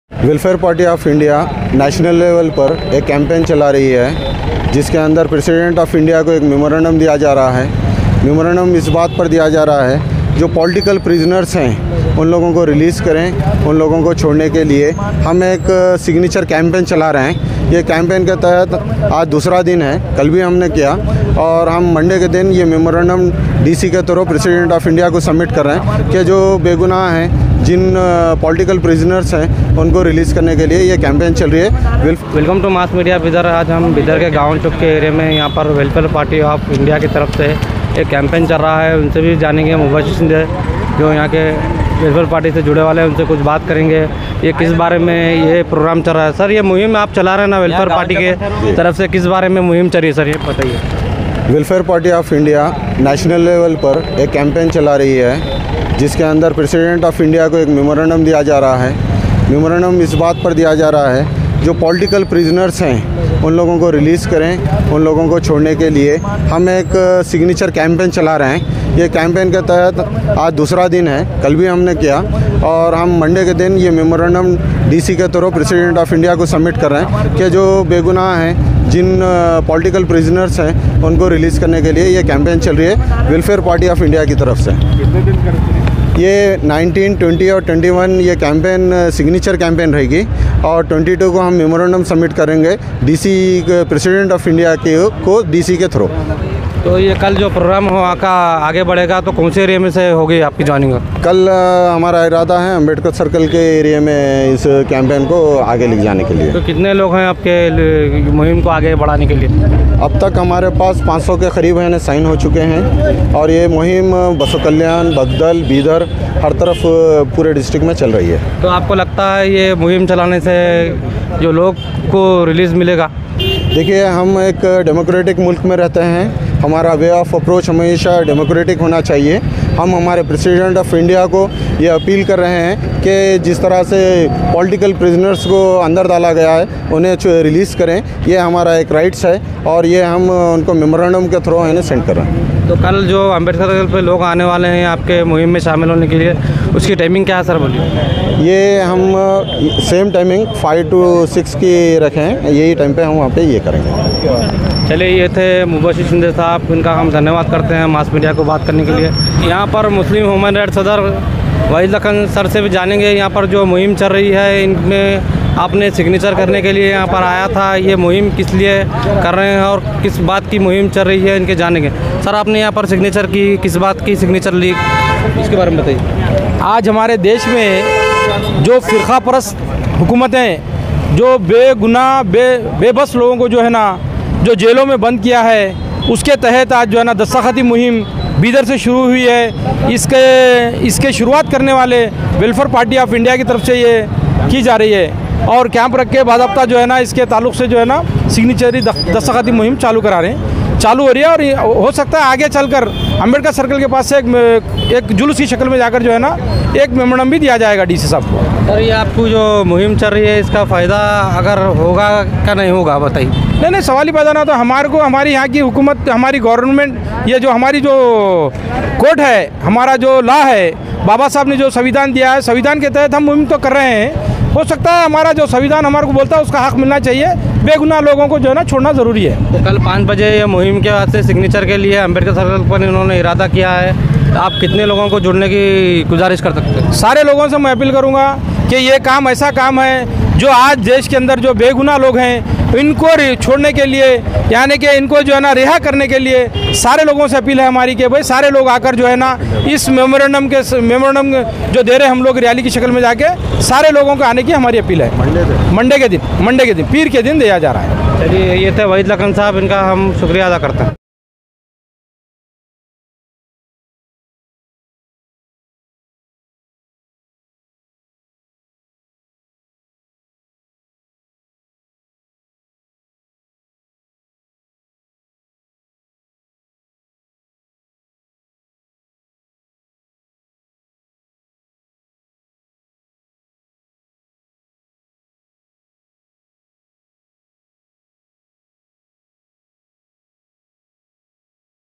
A Signature Campaign for the Released of Political Prisoners People who Are locked up in Jail
Welfare Party of India Bidar President Mubashir Sindhe And his Colleagues have launched a Signature Campaign for the Released of Political Prisoners People who Are locked up in Jail And today People Were Seen Signing Signatures at Gawan Chowk Bidar.